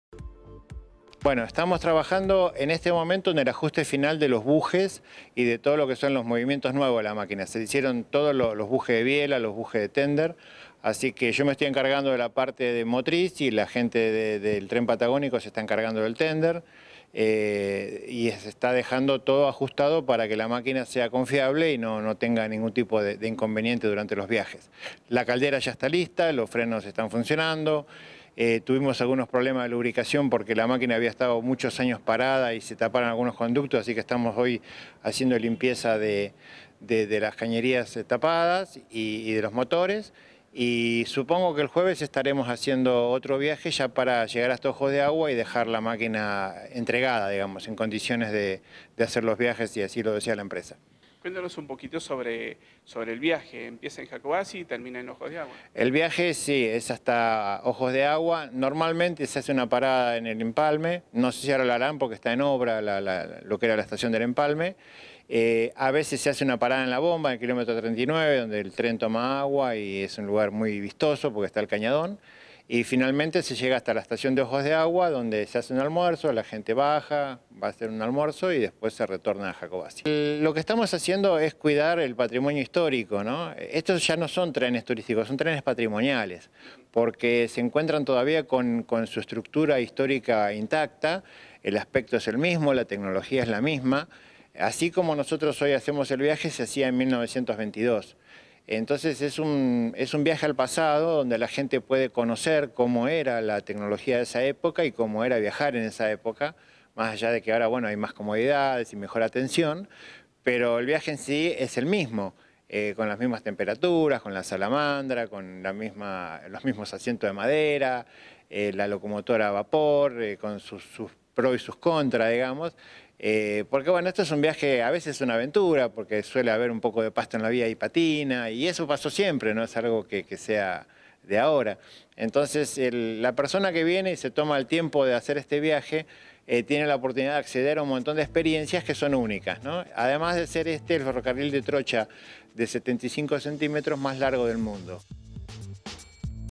Noticias10 | El 23 de enero, vuelve La Trochita
La pintoresca "Trochita" volverá a transitar la estepa rionegrina a partir del próximo sábado 23 de enero, con una excursión única y fascinante para turistas y residentes.